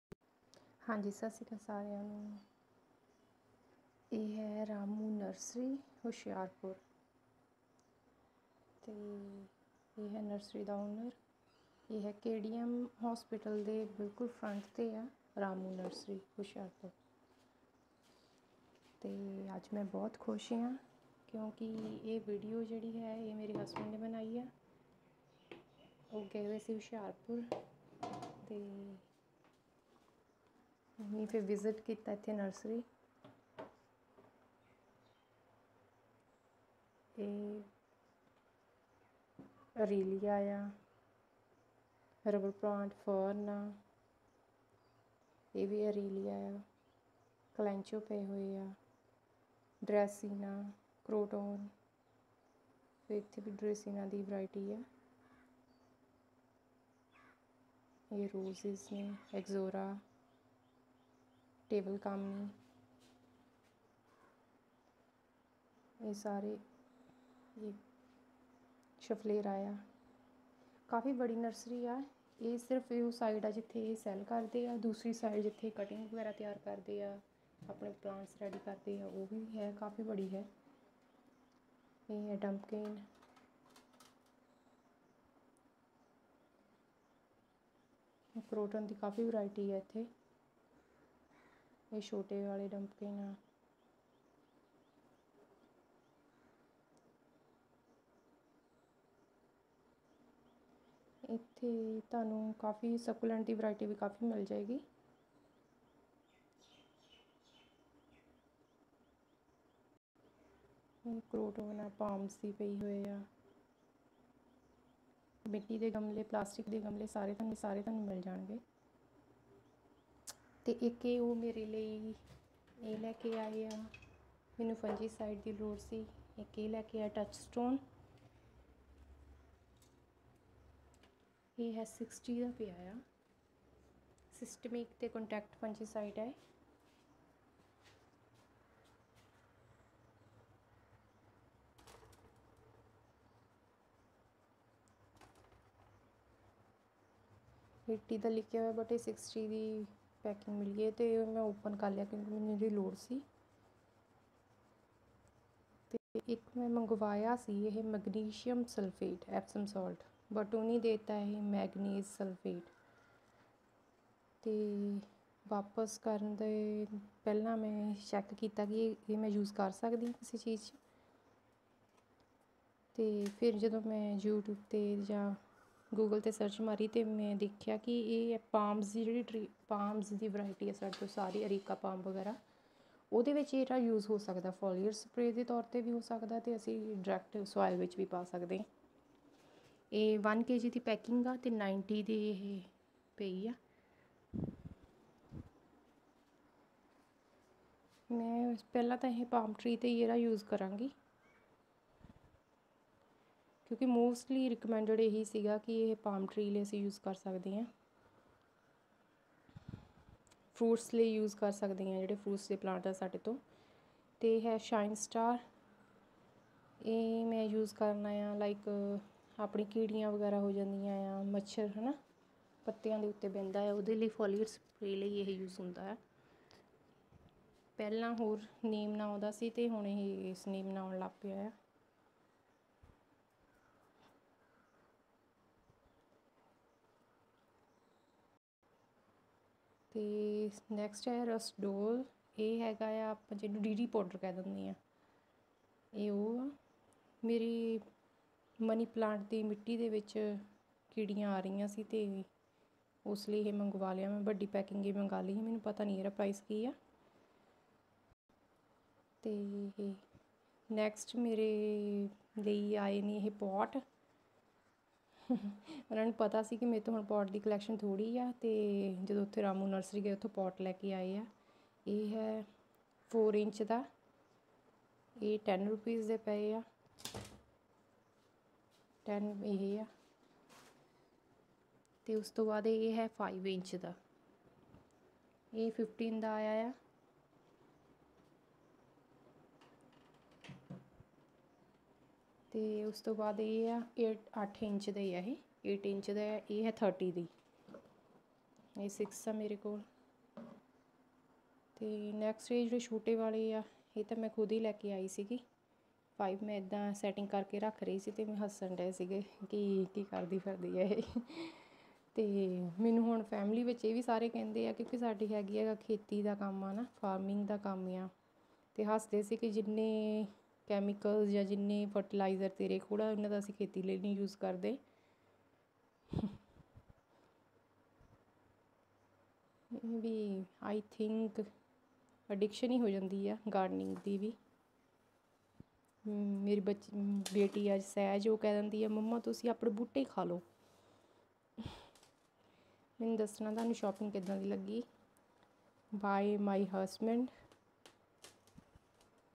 Nursery visit,Ramu nursery hoshiarpur, bought some pots and fertilizer
Ramu nursery hoshiarpur#support farmers 🙏🌾